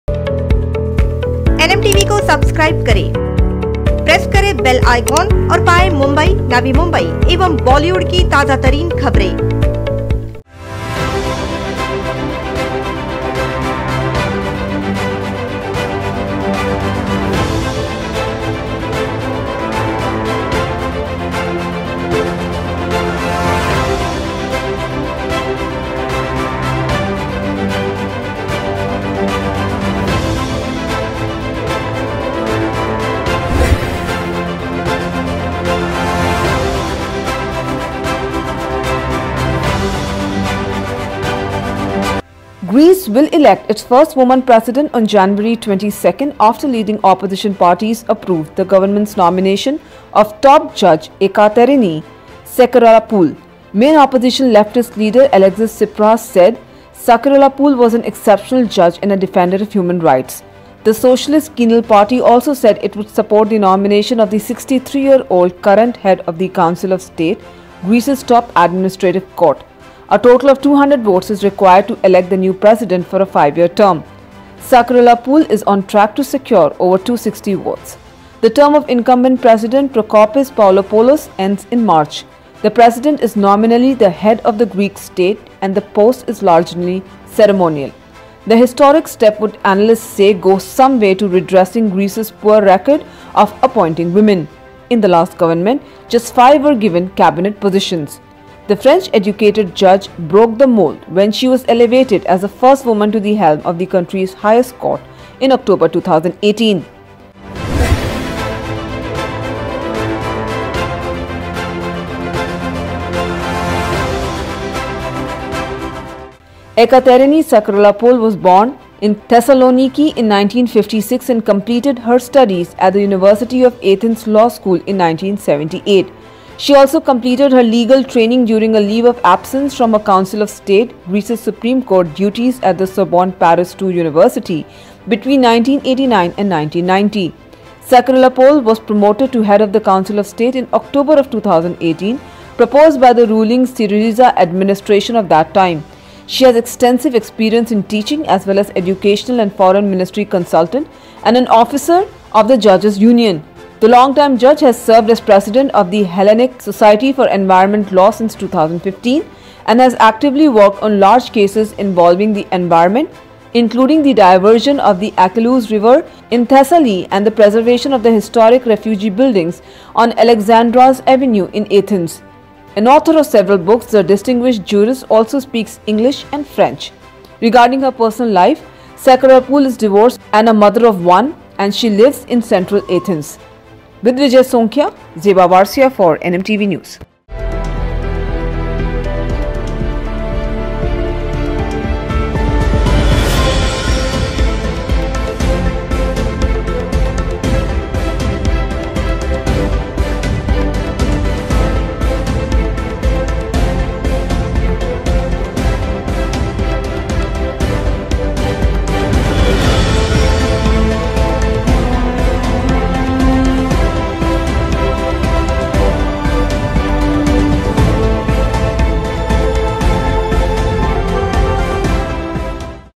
Ekaterini Sakellaropoulou to become Greece's First Woman President on January 22
Greece will elect its first woman President on January 22 after leading opposition parties approved the government's nomination of top judge Ekaterini Sakellaropoulou. Main opposition leftist leader Alexis Tsipras said Sakellaropoulou was an "exceptional judge" and a defender of human rights. The socialist KINAL party also said it would support the nomination of the 63-year-old current head of the council of state, Greece's top administrative court. A total of 200 votes is required to elect the new President for a five-year term. Sakellaropoulou is on track to secure over 260 votes. The term of incumbent President Prokopis Pavlopoulos ends in March. The President is nominally the head of the Greek state and the post is largely ceremonial. The historic step would, analysts said, go some way to redressing Greece’s poor record of appointing women. In the last government, just five were given cabinet positions. The French-educated judge broke the mould when she was elevated as the first woman to the helm of the country’s highest court in October 2018.

WHO IS EKATERINI SAKELLAROPOULOU?
Ekaterini Sakellaropoulou was born in Thessaloniki in 1956 and completed her studies at the University of Athens Law School in 1978. She also completed her legal training during a leave of absence from her Council of State, Greece’s Supreme Court, duties at the Sorbonne, Paris II University between 1989 and 1990. Sakellaropoulou was promoted to head of the Council of State in October of 2018, proposed by the ruling SYRIZA administration of that time. She has extensive experience in teaching, as well as an educational and Foreign Ministry consultant, and as an officer of the judges’ union. The longtime judge has served as President of the Hellenic Society for Environmental Law since 2015, and has actively worked on large cases involving the environment, including the diversion of the Acheloos River in Thessaly and the preservation of the historic refugee buildings on Alexandras Avenue in Athens. An author of several books, the distinguished jurist also speaks English and French. Regarding her personal life, Sakellaropoulou is divorced and a mother of one, and she lives in central Athens. With Vijay Sonkhia, Zeba Warsia for NMTV News.